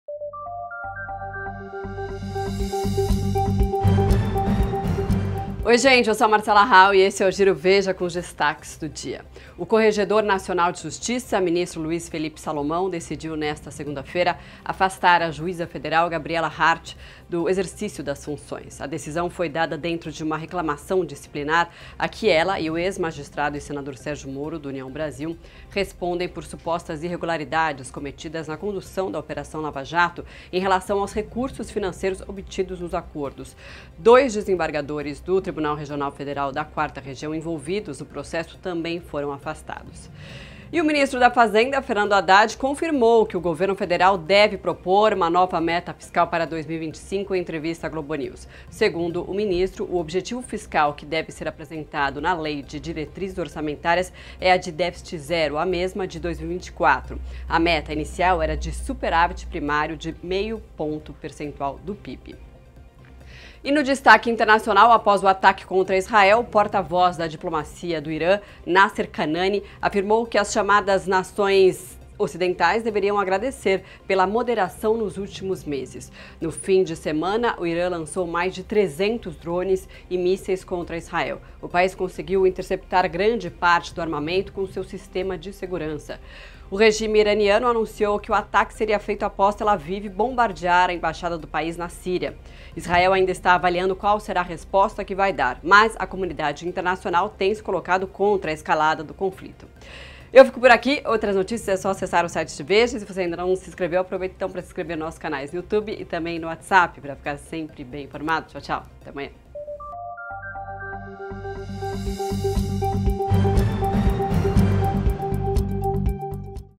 Juíza da Lava Jato afastada e a tensão no Oriente Médio | Giro VEJA
O corregedor nacional de Justiça, ministro Luís Felipe Salomão, decidiu nesta segunda-feira afastar a juíza federal Gabriela Hardt do exercício das funções. A decisão foi dada dentro de uma reclamação disciplinar a que ela e o ex-magistrado e senador Sergio Moro, do União Brasil, respondem por supostas irregularidades cometidas na condução da operação Lava-Jato, em relação aos recursos financeiros obtidos nos acordos. O afastamento da juíza, a nova meta fiscal do governo e o ataque do Irã contra Israel são destaques do Giro VEJA.